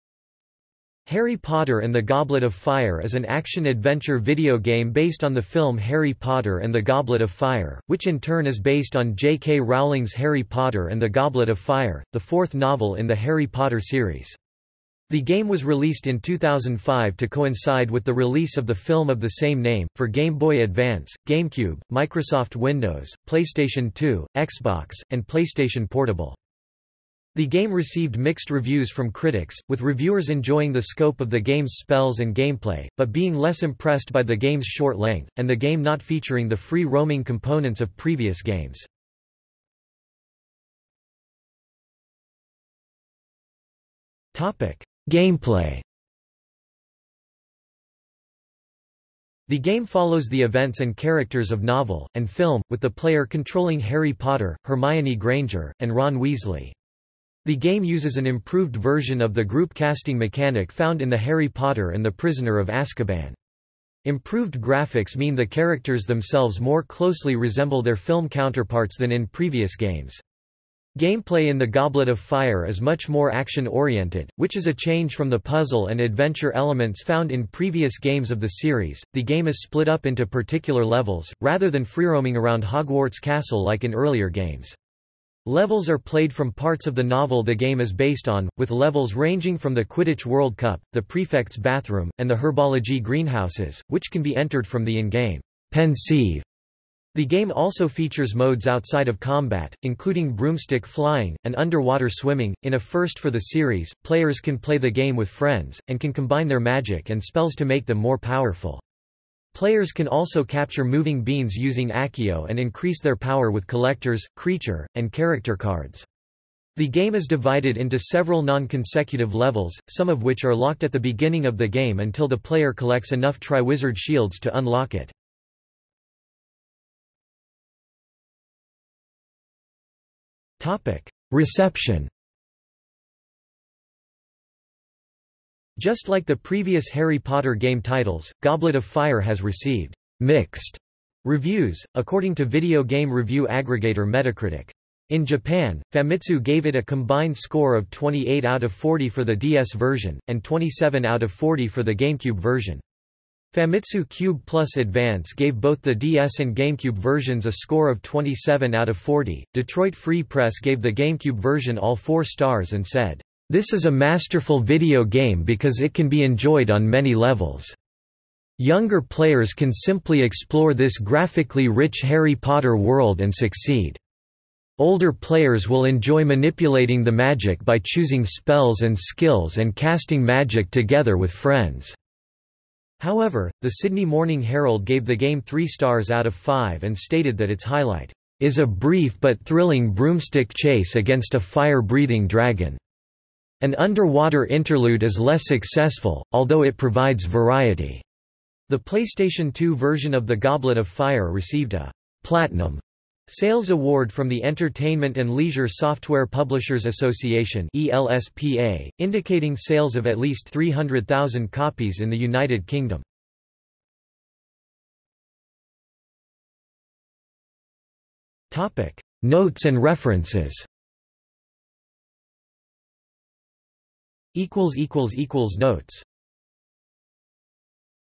Harry Potter and the Goblet of Fire (video game) | Wikipedia audio article
00:00:40 1 Gameplay
00:02:07 2 Reception
00:03:34 3 Notes and References

- Socrates

SUMMARY
Harry Potter and the Goblet of Fire is an action-adventure video game based on the film Harry Potter and the Goblet of Fire, which in turn is based on J. K. Rowling's Harry Potter and the Goblet of Fire, the fourth novel in the Harry Potter series. The game was released in 2005 to coincide with the release of the film of the same name, for Game Boy Advance, GameCube, Microsoft Windows, PlayStation 2, Xbox, and PlayStation Portable.
The game received mixed reviews from critics, with reviewers enjoying the scope of the games spells and gameplay, but being less impressed by the game's short length, and the game not featuring the free roaming components of previous games.